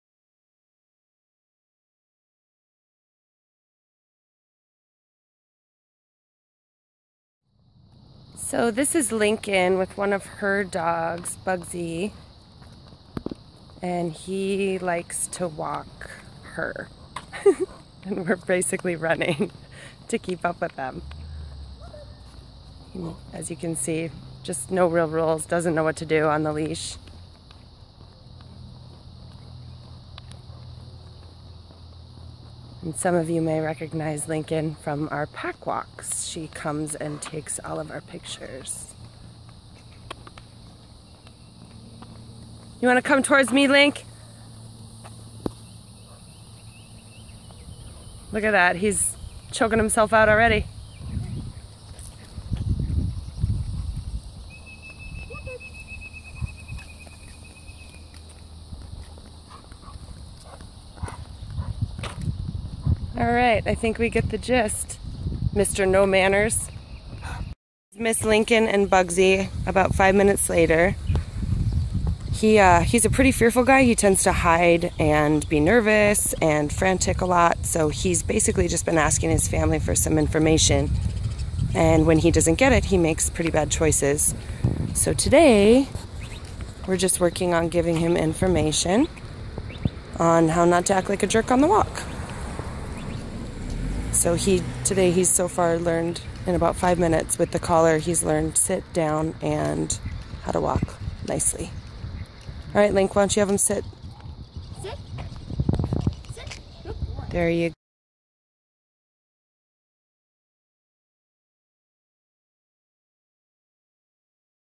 Before and After with a Nervous Coonhound/Ridgeback Mix
Bugsy is a super fearful dog and was an absolute mess on the leash pulling his humans all over the place in an attempt to "flee" all the scary stuff on walks. His haphazard walk is indicative of his overall mind set. chaos! Watch him transform with some guidance in just one session!